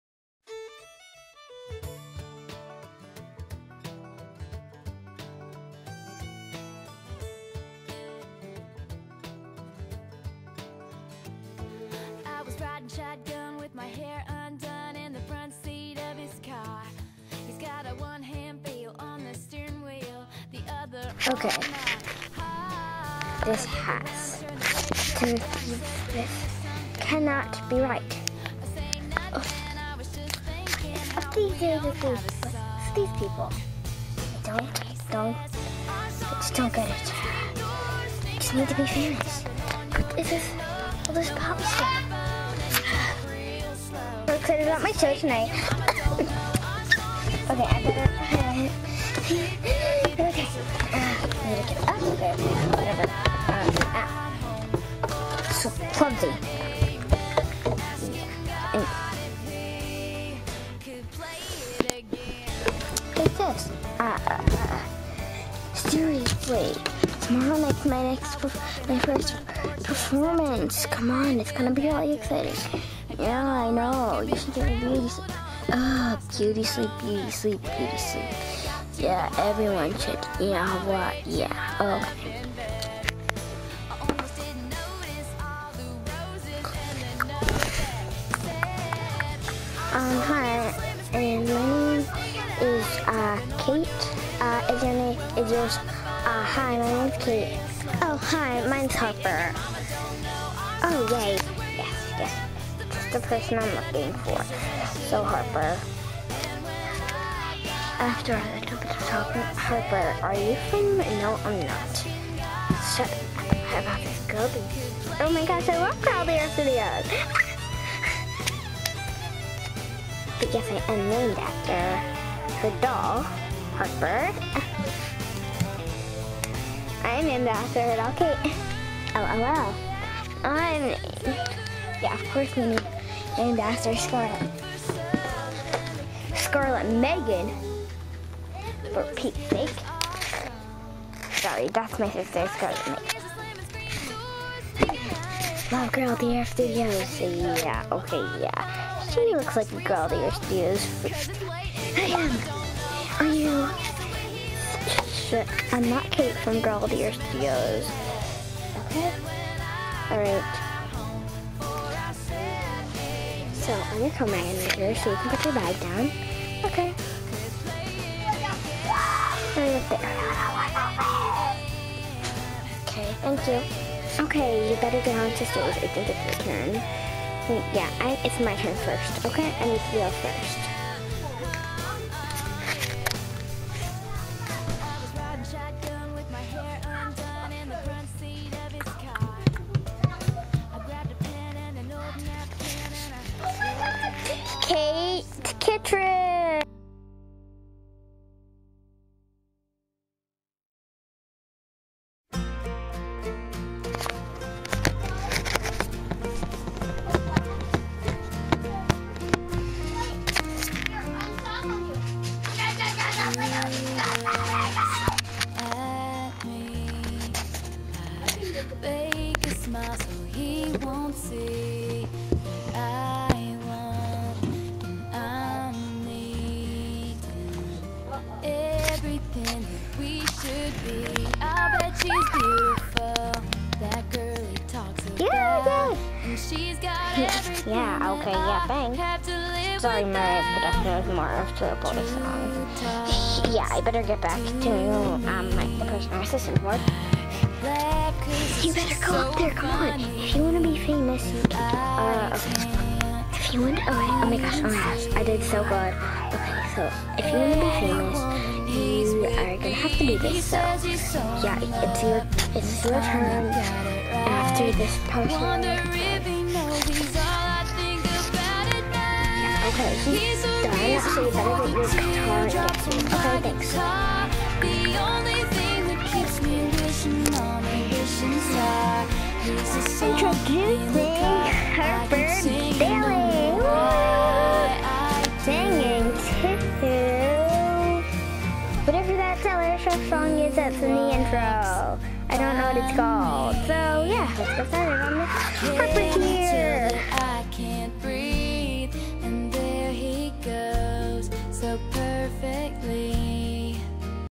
Olympic Singer Part 1/2
AG doll series based on GOTYS Music In Me. im gotys obsessed. Ali (gotys please comment